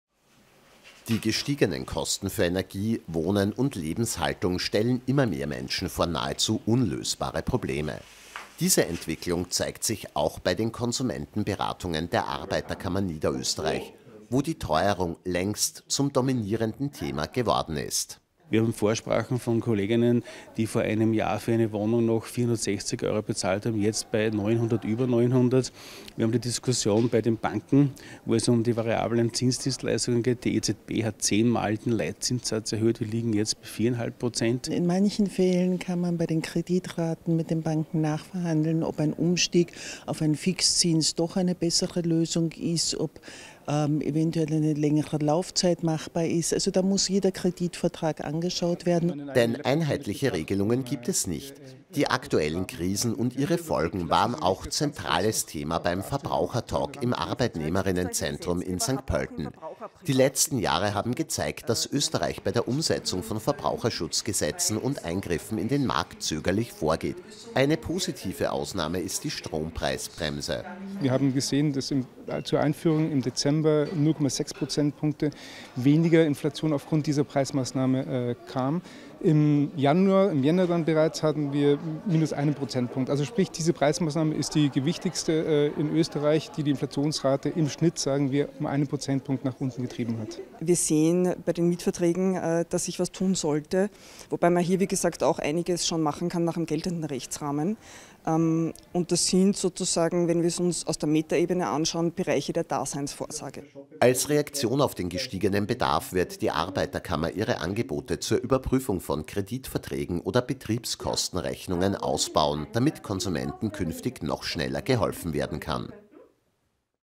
Verbraucherschutz in Krisenzeiten
Die Krisen der letzten drei Jahre haben bei vielen Menschen deutliche Spuren im Haushaltsbudget hinterlassen und viele können sich die gestiegenen Kosten für Energie, Wohnen und Lebenshaltung nicht mehr leisten. Diese Entwicklung zeigt sich auch bei den Konsumentenberatungen der Arbeiterkammer Niederösterreich. Bei einem „Verbrauchertalk“ im Arbeitnehmerinnenzentrum in St.Pölten haben Vertreter der Arbeiterkammer nun gemeinsam mit namhaften ExpertInnen über die Hintergründe und mögliche Auswege diskutiert.